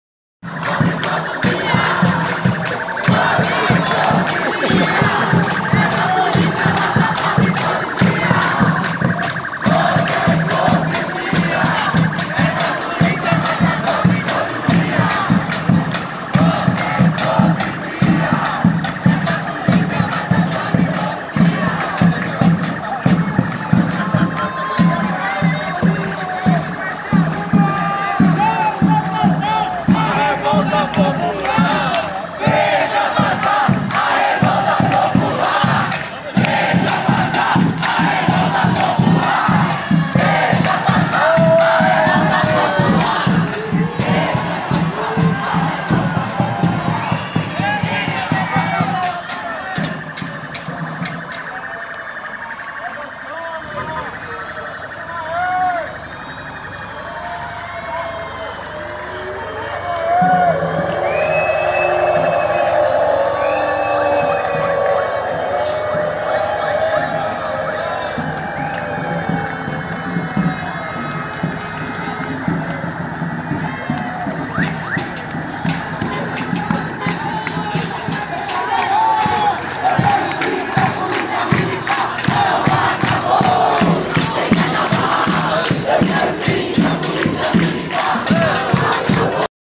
Caça ao prefeito foragido!
3º Grande Ato: Caça ao prefeito foragido! & Lançamento do vídeo Novo Apocalipse Recife, do Movimento Ocupe Estelita
Saida: Praça Cidade do Porto - Boa Viagem, Recife